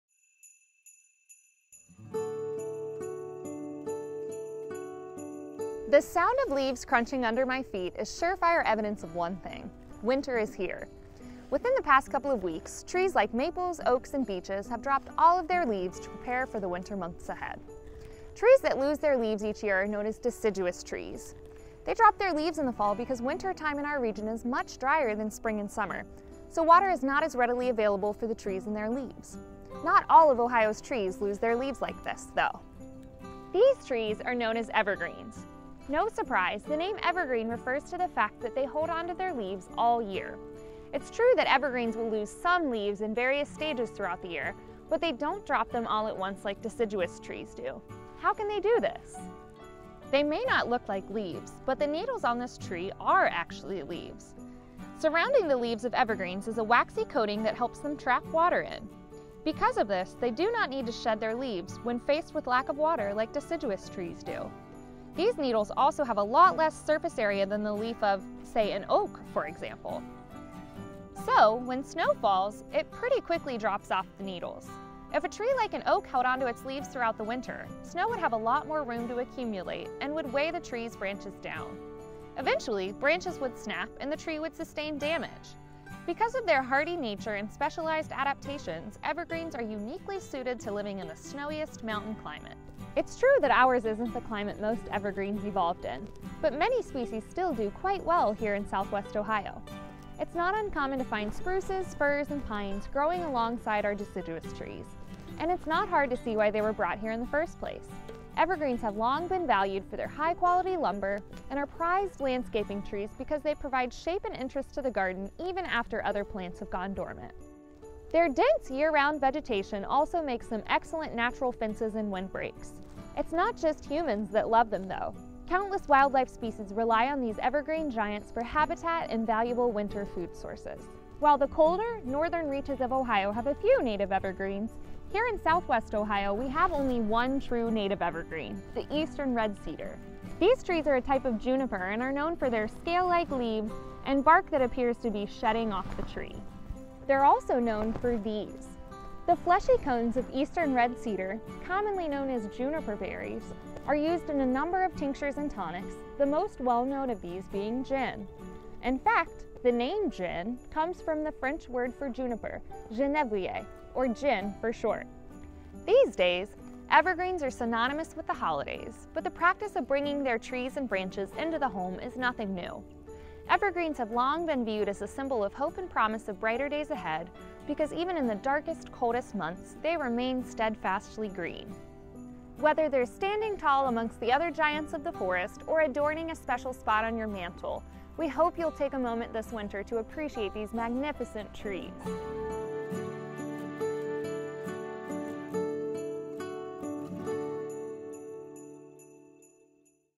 Evergreens: The Nature of a Holiday Tradition
Evergreens have long been viewed as a symbol of hope and promise of brighter days ahead, because even in the darkest, coldest months, they remain steadfastly green. Although evergreens evolved to withstand snowy mountain climates, we do have one native evergreen in southwest Ohio, the Eastern Redcedar, and there are many other species that have been brought to the area. Learn more about the natural history of the trees behind holiday traditions!